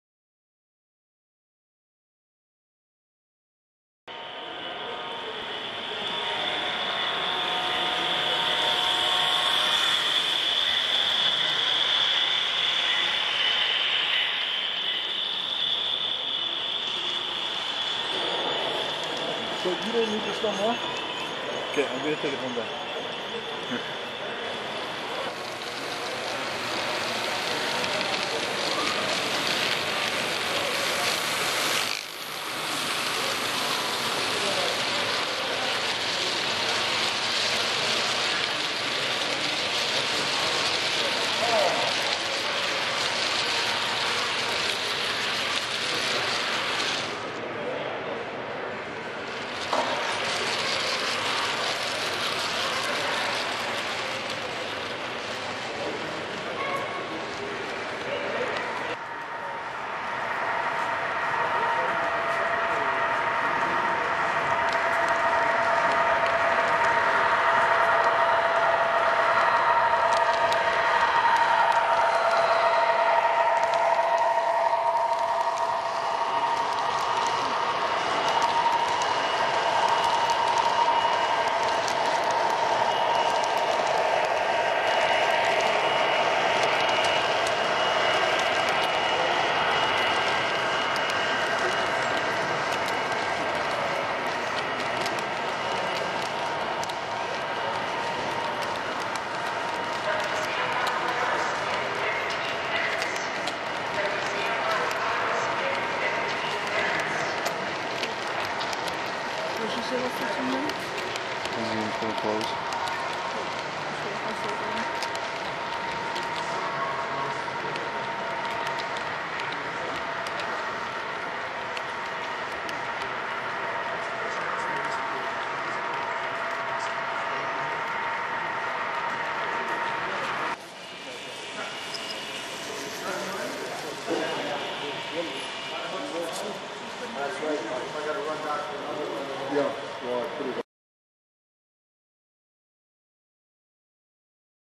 Moose Jaw Saskatchewan Train Show 2010
Train show in Moose Jaw, Saskatchewan at the Western Development Museum